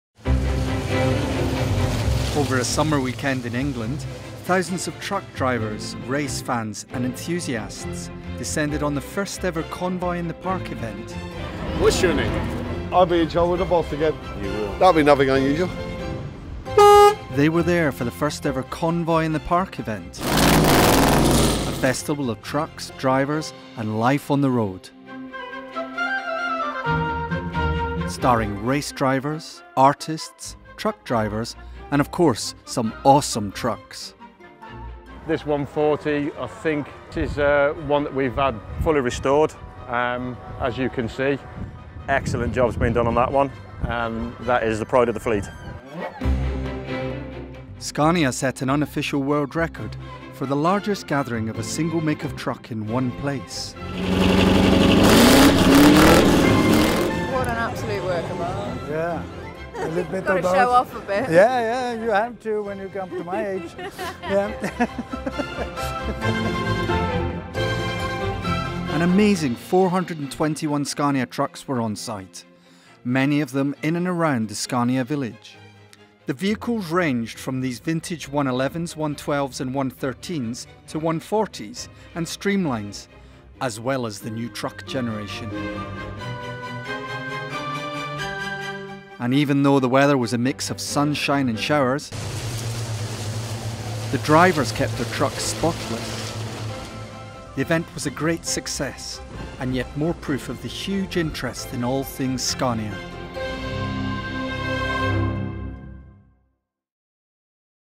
World record for Scania at Convoy in the Park
"An amazing 421 Scania trucks gathered in and around the Scania Village area of Convoy in the Park, making it the largest gathering of a single make of truck in one place. The huge array of trucks made for an impressive sight, and ranged from vintage-era 110s through 140s to the New Truck Generation. The prize for Best Scania truck in the village went to the V8 customised T-cab driven by Coles & Sons, and this truck also won the overall prize for best in show," says Scania Group